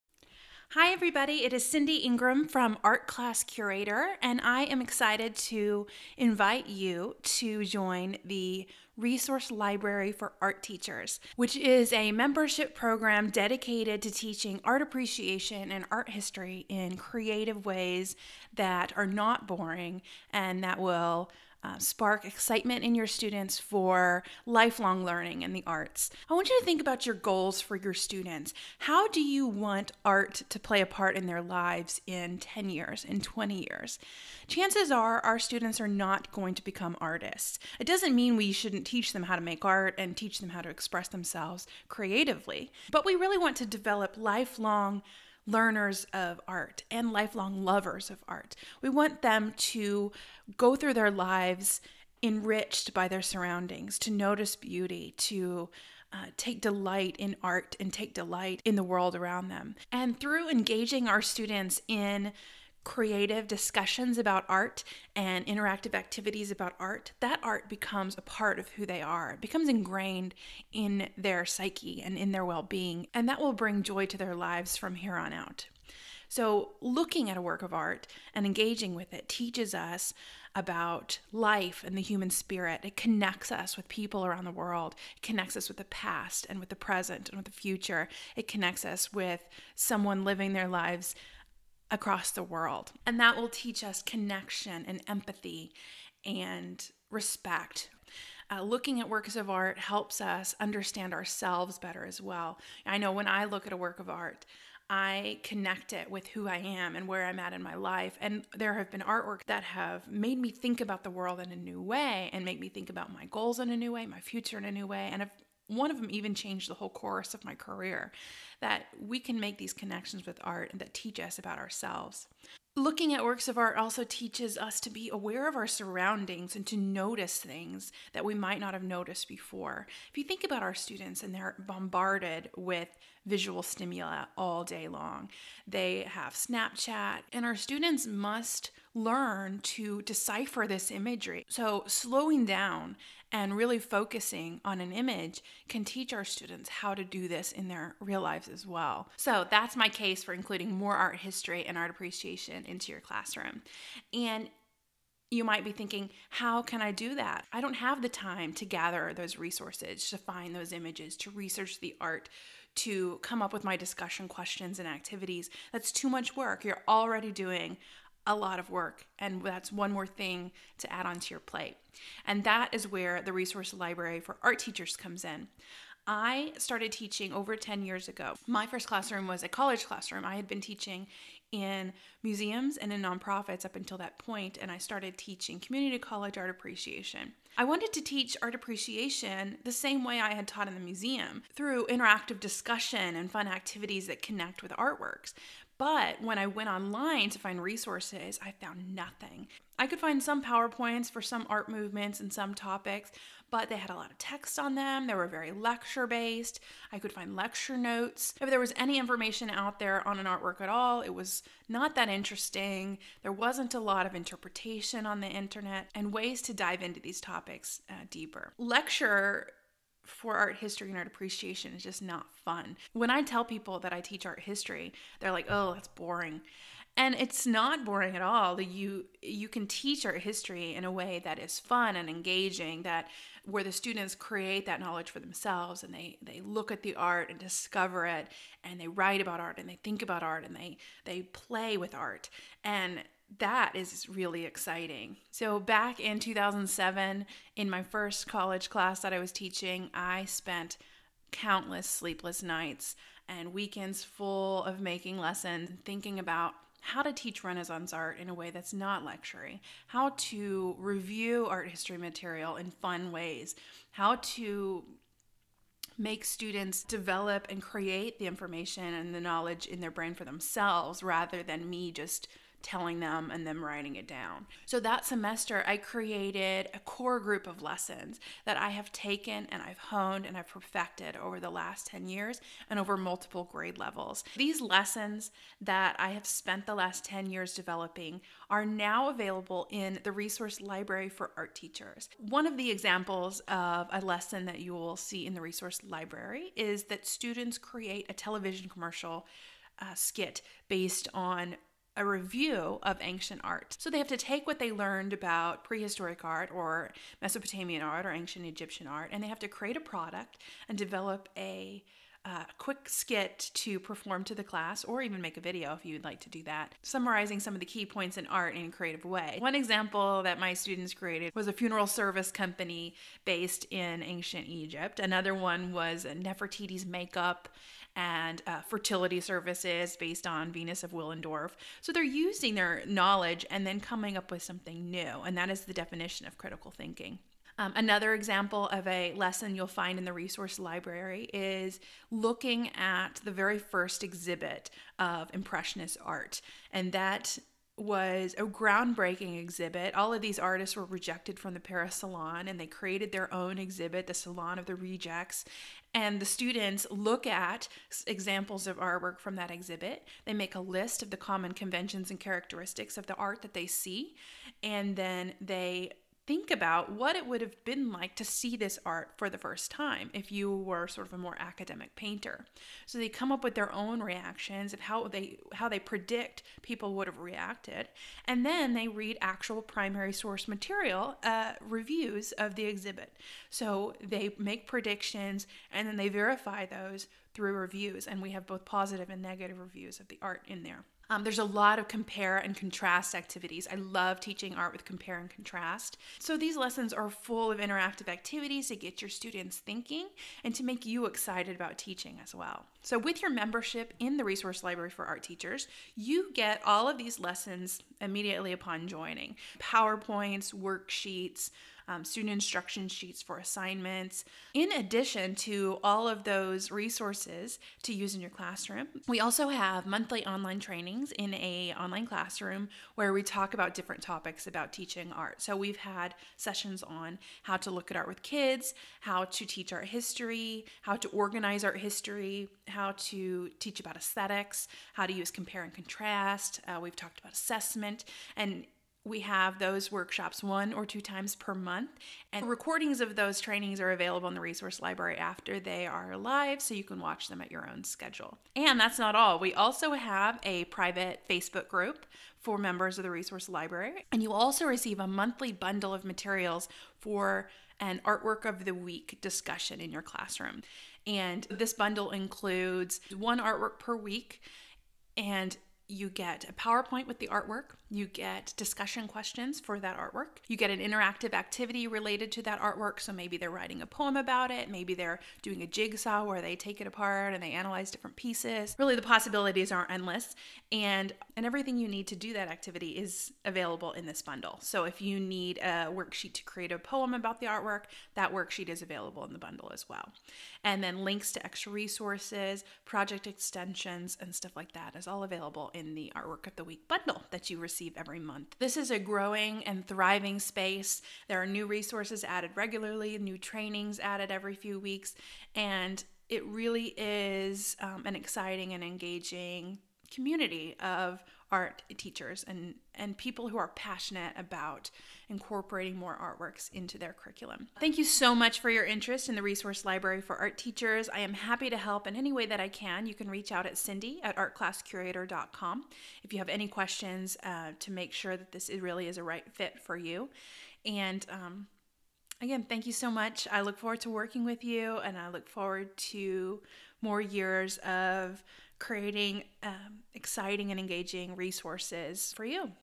Why Kids Need to Look at Art
The importance of looking at art with your students and the resources to make this happen!